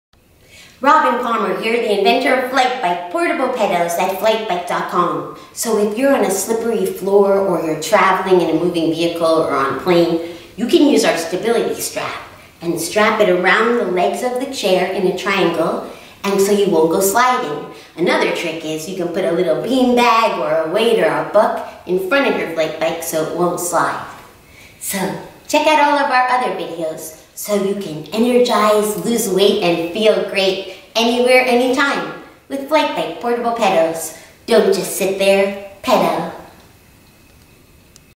FlyteBike Stability Strap
If you're on a slippery surface or a moving vehicle or plane, utilize the stability strap so your FlyteBike won't go flying off without you. Simply clip it around the legs of your chair and you're ready to go. You can also use a rubber mat or a weight on a slippery surface to keep your FlyteBike in place!